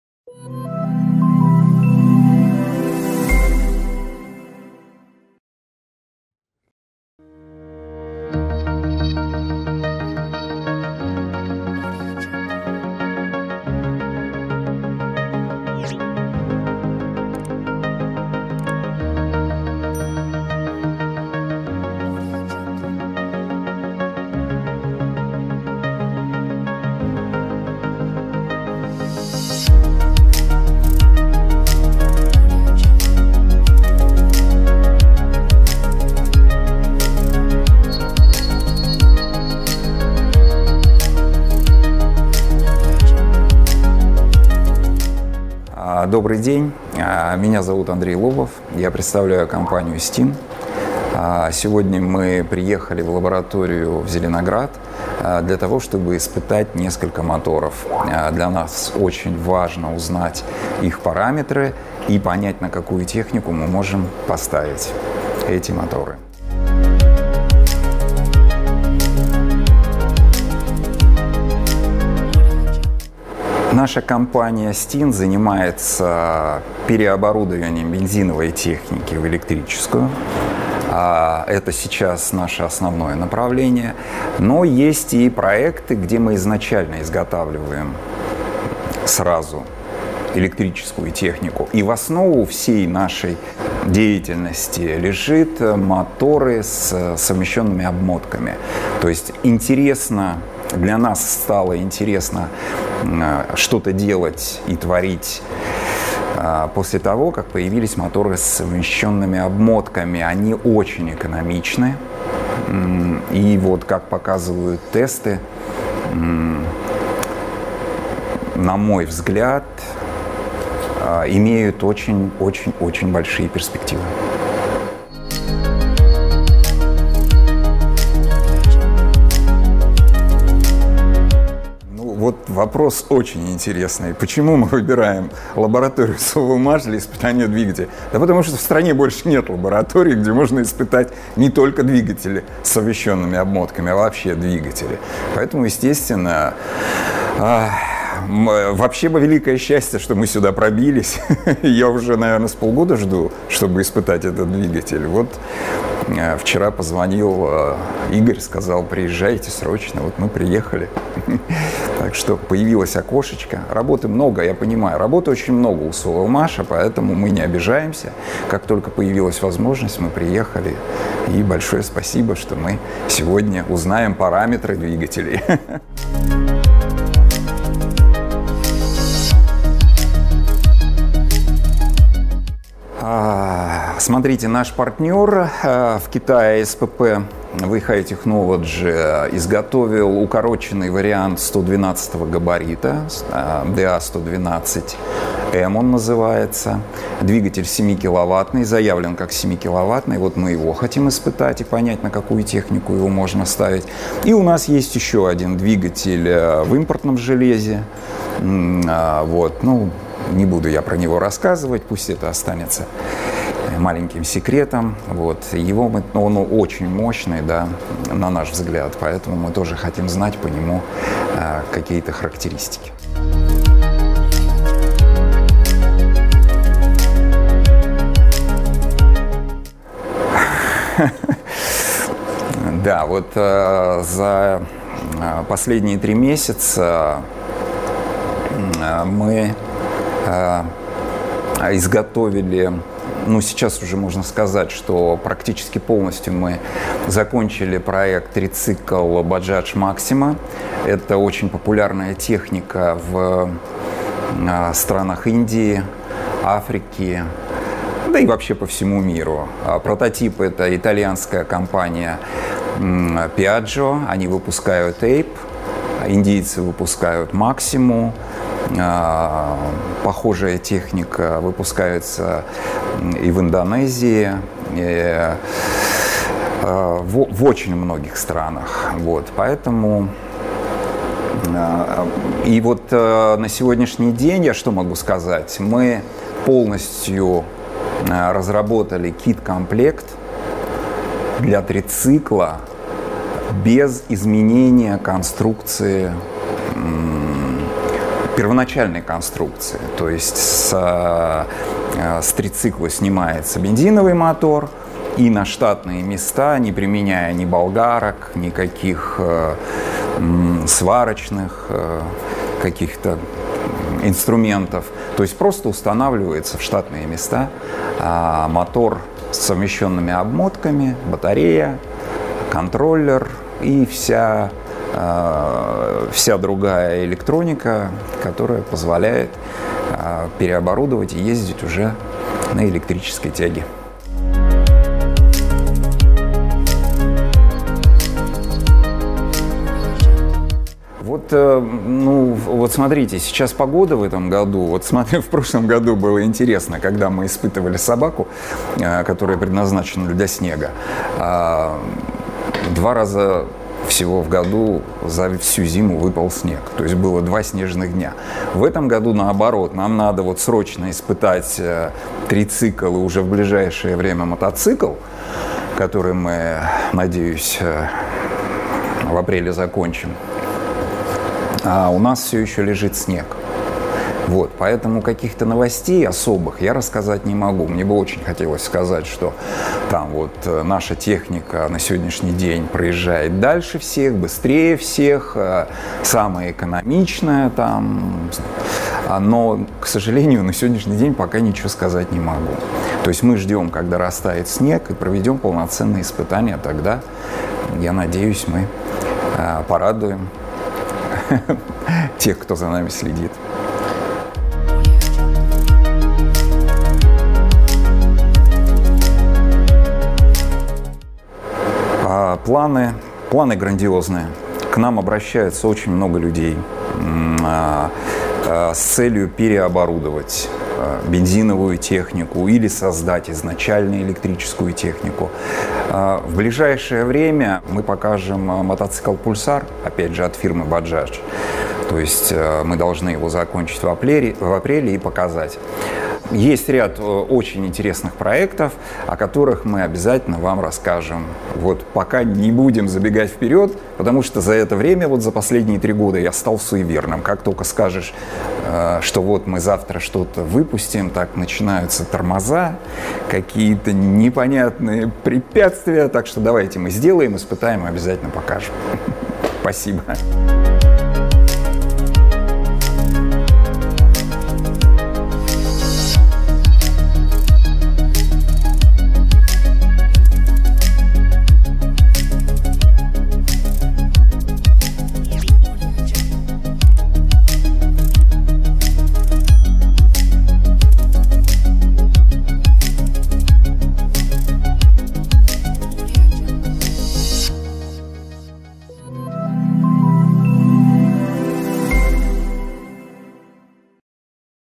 Испытание двигателей из Китая в лаборатории «Совэлмаш» l Андрей Лобов
Недавно лабораторию «Совэлмаш» посетили представители компании “СТиИН” с целью проведения испытаний электродвигателей. Основатель компании Андрей Лобов рассказал нам подробнее о деятельности “СТиИН” и цели своего приезда:

«Наша компания занимается переоборудованием бензиновой техники в электрическую, а также изготовлением электрической техники. В основе нашей деятельности — электродвигатели с совмещёнными обмотками, поскольку они экономичны и, как показывают тесты, имеют очень большие перспективы.

Приезд в лабораторию «Совэлмаш» мы планировали уже давно и очень рады, что он состоялся. Мы приехали сюда, чтобы испытать несколько электродвигателей. Нам важно узнать их параметры и понять, на какую технику мы можем их поставить. Один из них — укороченный вариант электродвигателя 112 габарита, который изготовил наш партнёр в Китае ASPP Weihai Technology. Что касается второго двигателя, пока мы не можем назвать его модель, но это двигатель зарубежного производителя, отличающийся достаточно высокой мощностью».

В данном видеоролике Андрей Лобов также расскажет о текущих проектах и ближайших планах “СТиИН”.